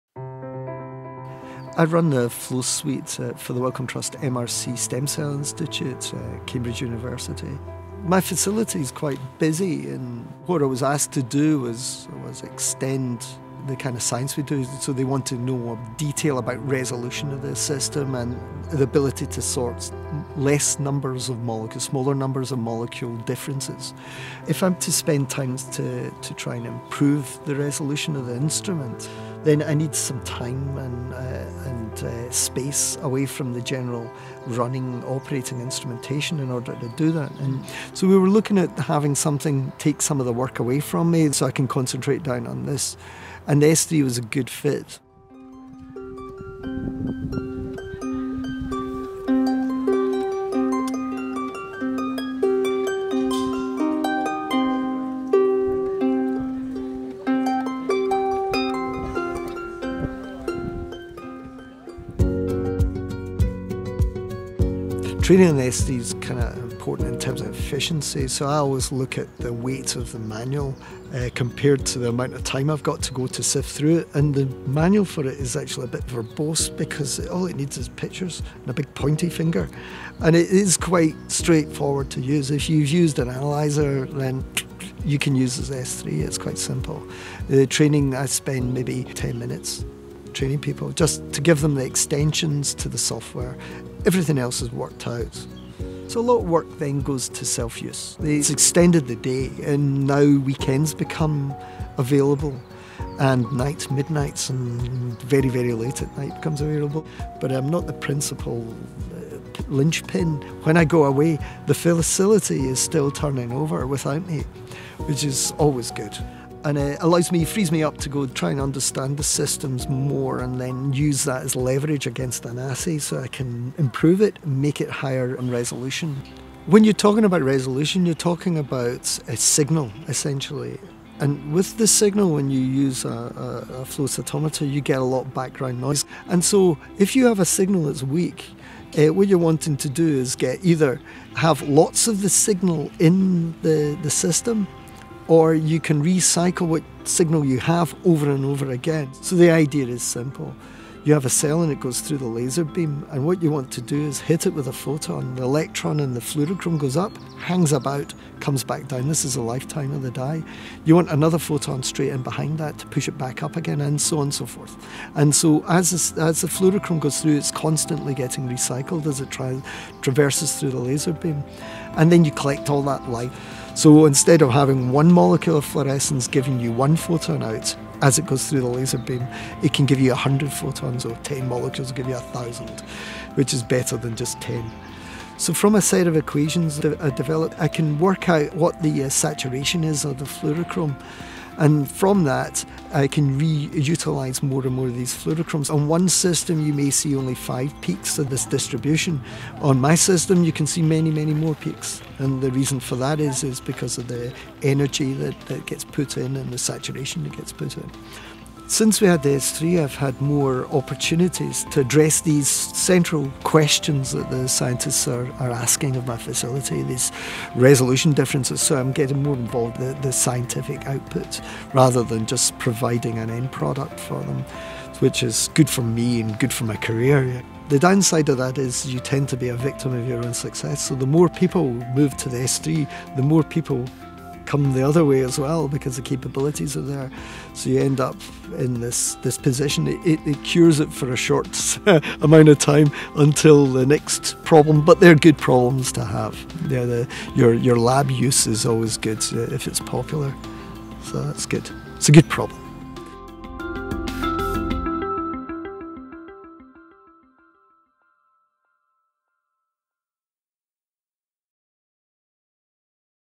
Expanding Flow Core Capabilities with the S3e™ Cell Sorter
Andy Riddell, Flow Cytometry Core Facility Manager at the Wellcome Trust- MRC Stem Cell Institutes, is expanding his core facility by allowing his researchers to conduct sorting on their own. In doing so, he is now free to explore and answer the challenging flow cytometry questions that require more of his expert time.  Find out how the S3e Cell Sorter has allowed Andy that freedom and how easy sorting can be for researchers.

Features and Benefits of the S3e Cell Sorter:
  • Hands-free nozzle tip alignment — features the fully automatic AutoGimbal™ System, driven completely by software
  • Simplified instrument setup — fully automated drop delay calculation, stream alignment, and droplet break-off monitoring enable precise 1–4 color sorting with minimal training
  • Compact design — dimensions of only 70 × 65 × 65 cm (2.3 × 2.1 × 2.1 ft) including onboard fluidics and temperature control system
  • 2-way cell sorting — sort two different defined populations at the same time
  • High speed and high purity — sort cells fast while maintaining high sensitivity and purity
  • Intuitive interface — user-friendly ProSort™ Software enables effortless instrument control and sort logic definition